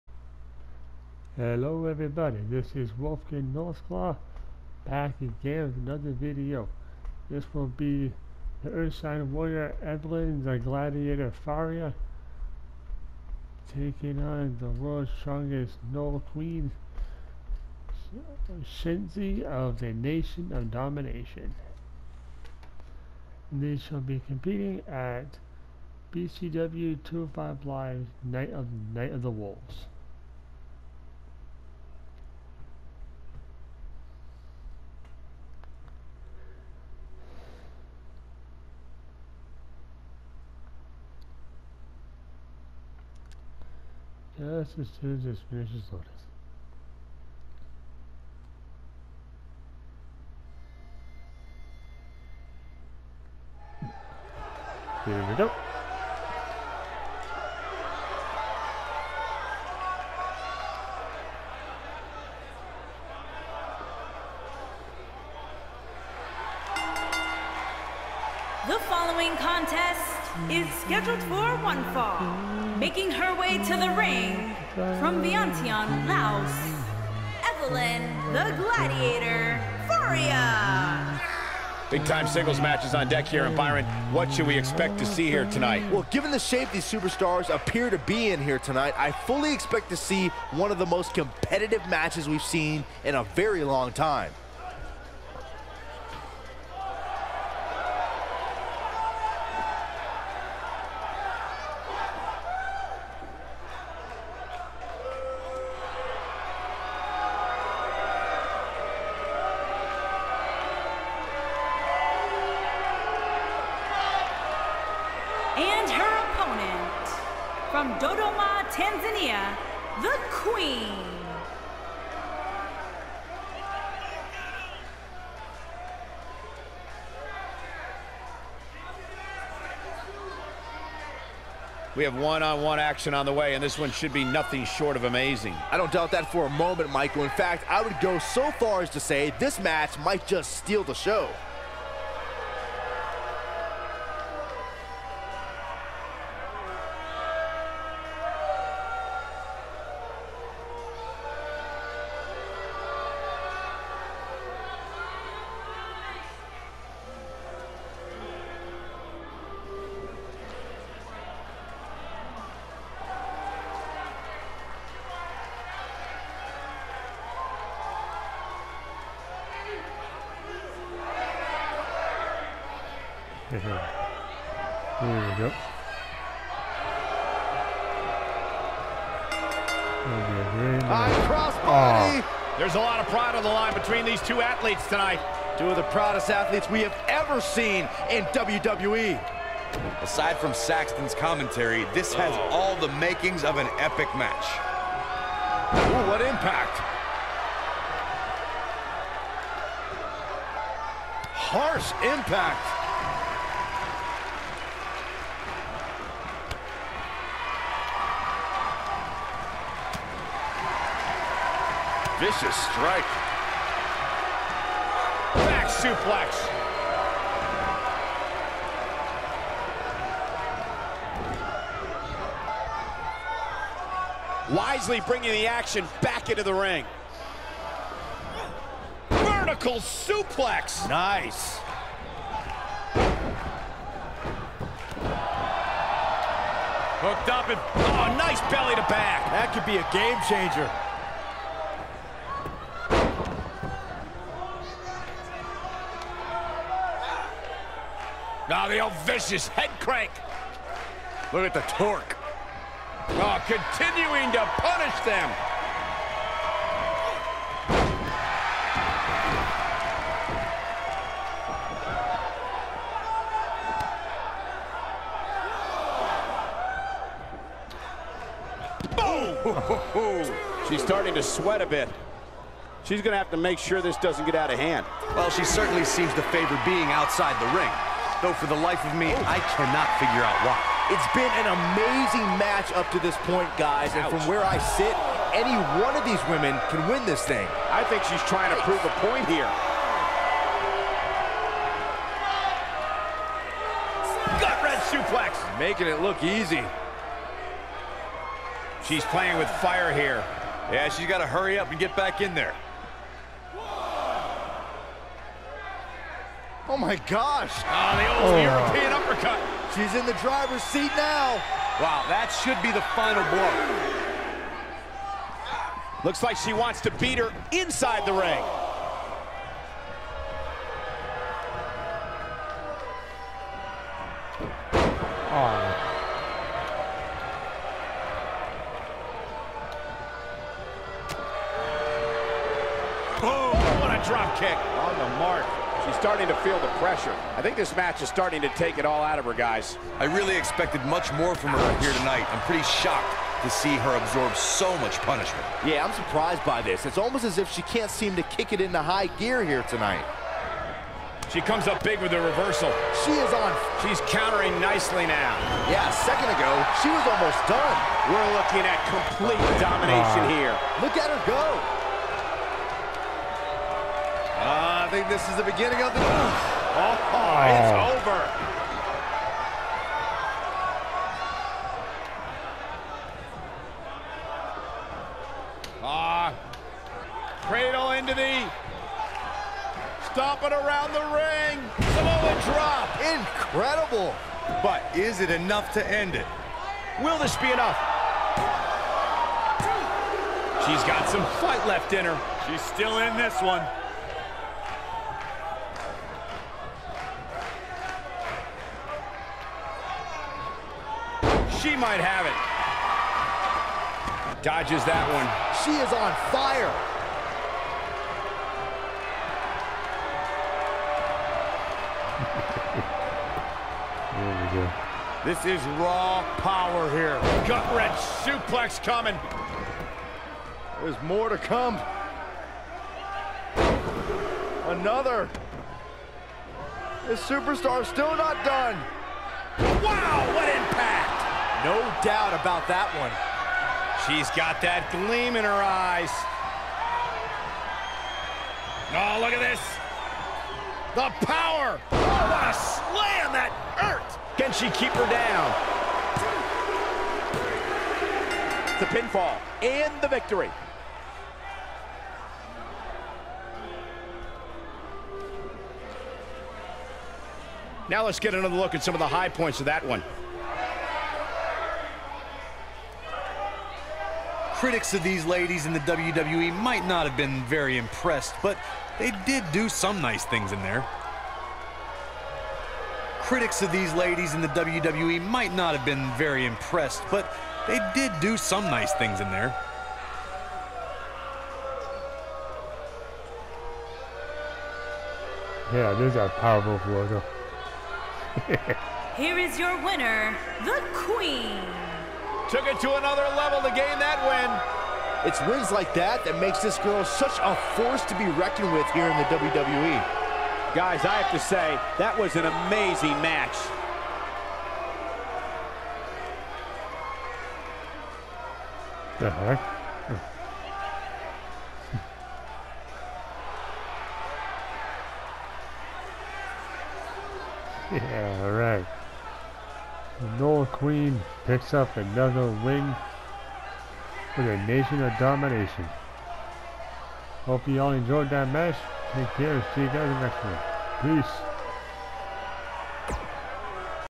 Evelyn "The Gladiator" Faria Vs Shenzi - Play Mode
The ursine warrior Evelyn "The Gladiator" Faria takes on the world strongest Gnoll queen Shenzi of The Nation of Domination.